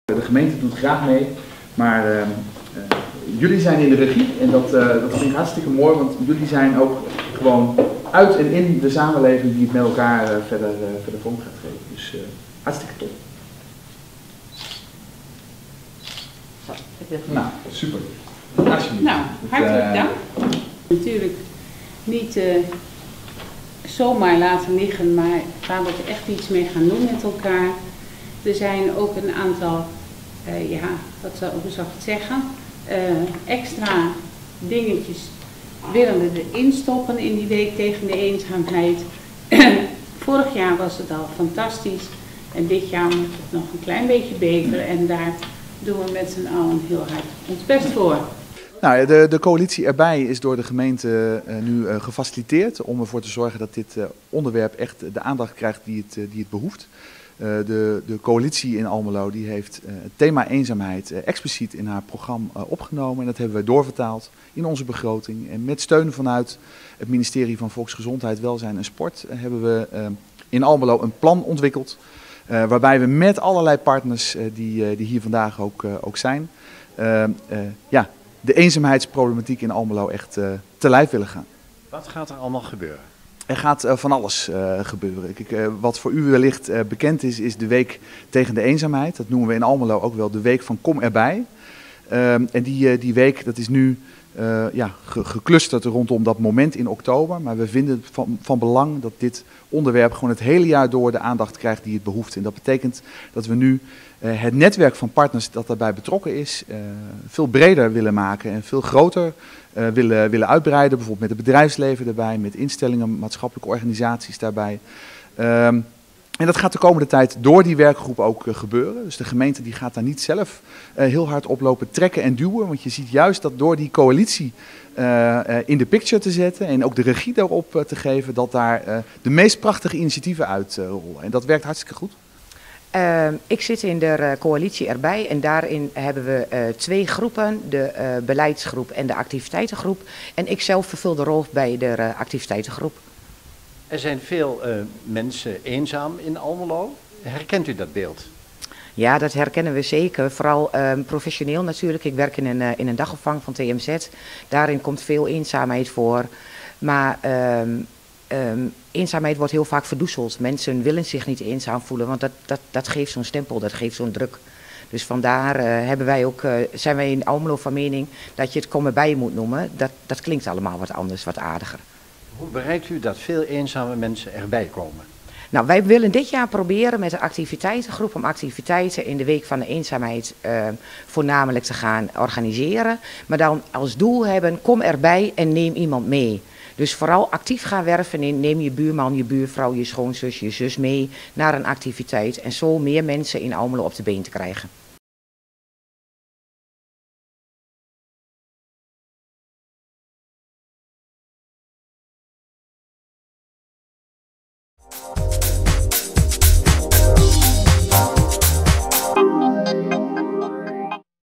Coalitie Erbij Almelo bestrijdt eenzaamheid
Kom erbij. Dat willen de gemeente Almelo en zo'n tien lokale partners duidelijk maken aan iedereen die zich eenzaam voelt. Of momenten kent van eenzaamheid. Ruim 30.000 inwoners van Almelo voelen zich soms eenzaam. Voor kortere tijd, soms voor langere tijd.